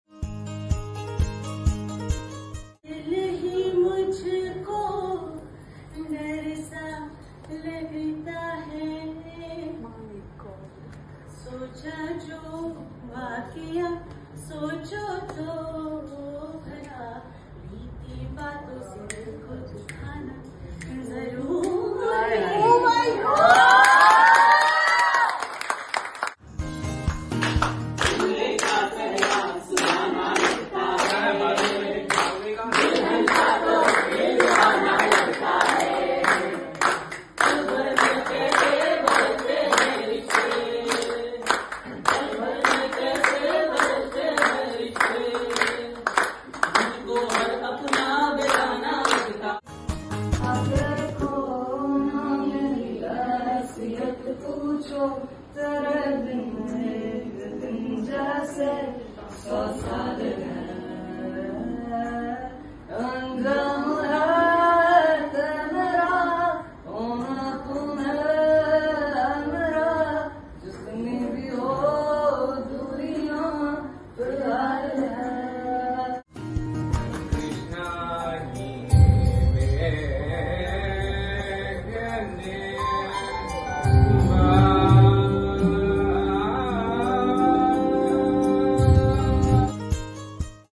Secret Star of Team Aeliya
🎥✨ We're excited to share a special moment here! Watch hidden talent take the stage for solo performances of their favorite songs. 🎵 From soulful melodies to energetic beats, each performance is a testament to their individual talent and passion for music. 🎤🎶